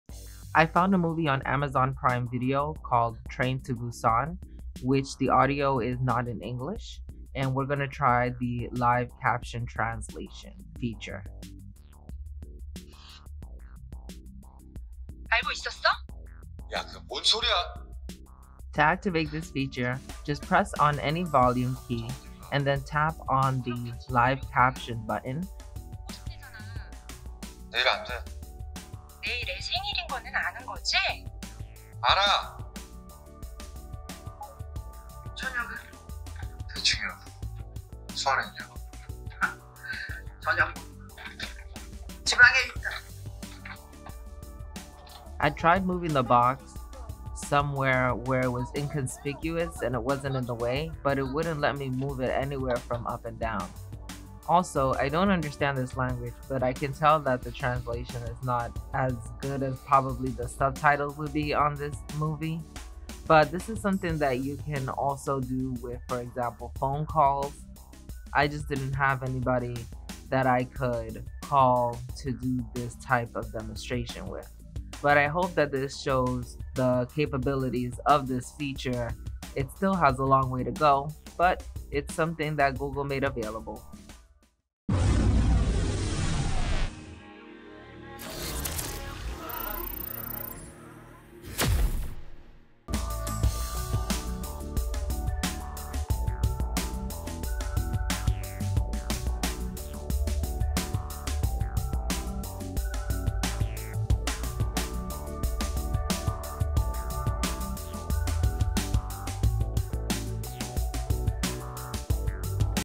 Live Caption with Translate On My Google Pixel 6 Pro
In this demonstration, I use my Google Pixel 6 Pro to show you how to activate Live Caption Translate Feature. It's pretty cool, live caption will detect that the audio is in another language and then translate and transcribe it in your language. It still needs some fixes, like being able to move it to an obscure place on the display and the translation is not as good as Live Translate.

Pixel 6 Pro
Pixel 6